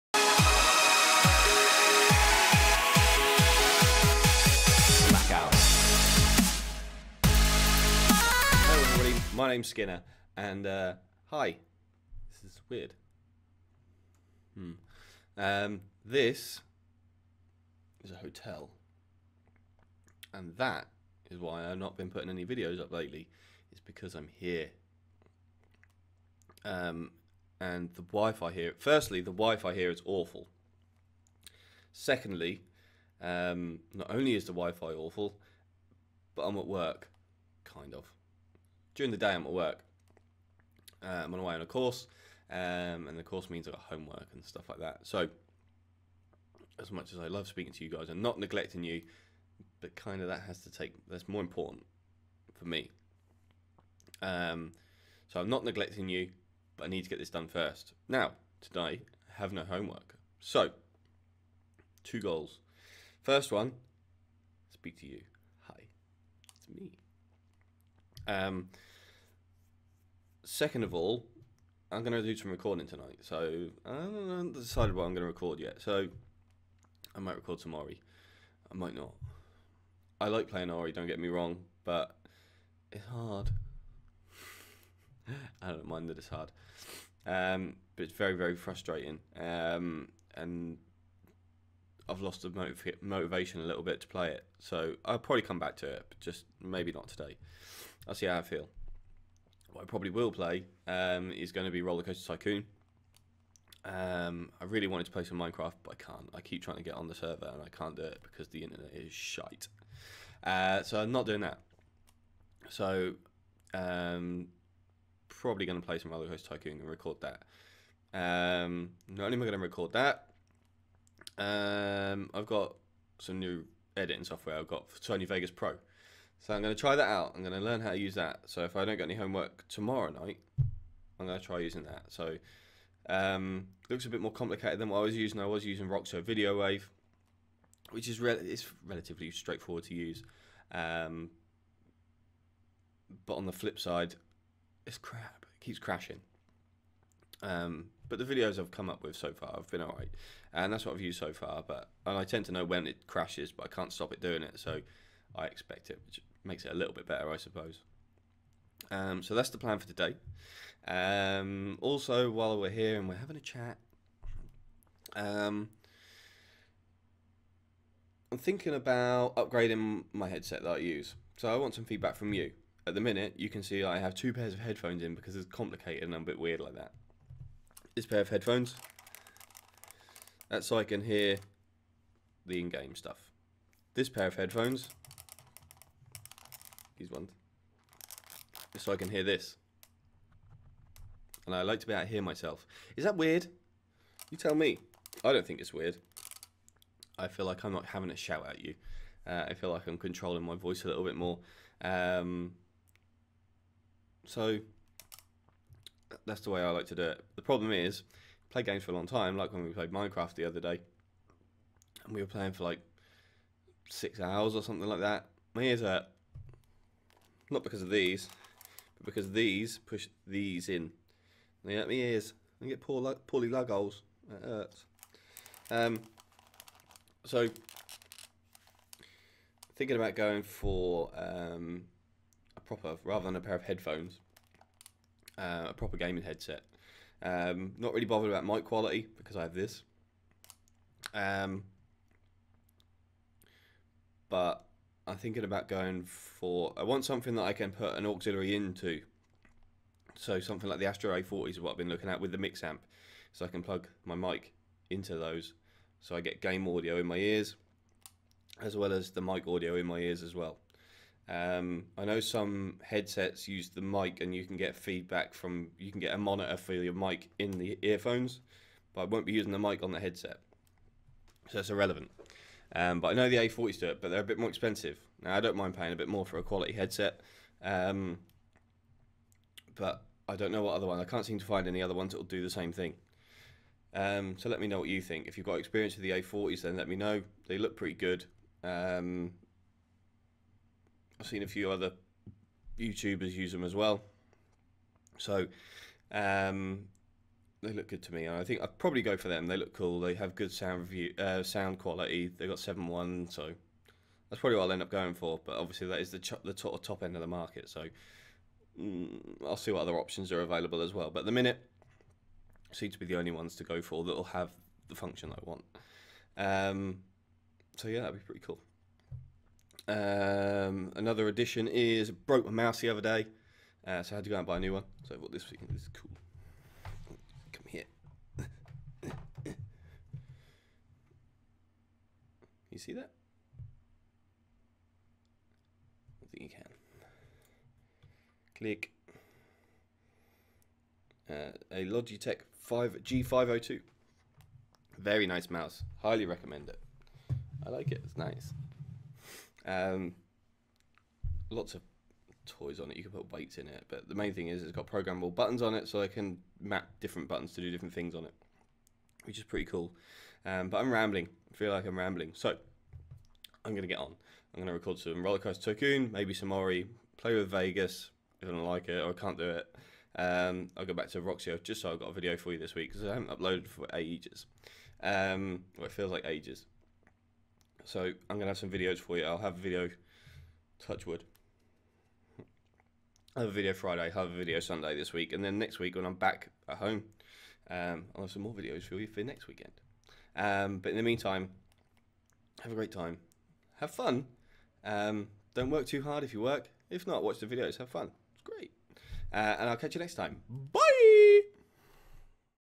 Awful WIFI And I Still Need To Stop Saying 'ERM'
Just thought i would say hi and let you know whats going on, ive not forgotten about you :)